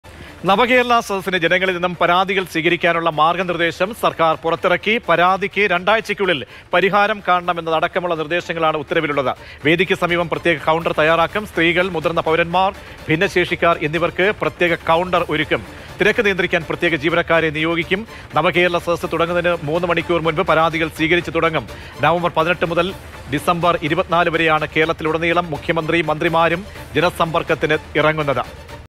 നവകേരള സദസ്സ്; ജനങ്ങളിൽ നിന്ന് പരാതികൾ സ്വീകരിക്കാനുള്ള മാർഗനിർദേശം പുറത്തിറക്കി | Navakerala Sadas
നവകേരള സദസിന് ജനങ്ങളിൽ നിന്ന് പരാതികൾ സ്വീകരിക്കാനുള്ള മാർഗനിർദേശം സർക്കാർ പുറത്തിറക്കി

Happy viewing!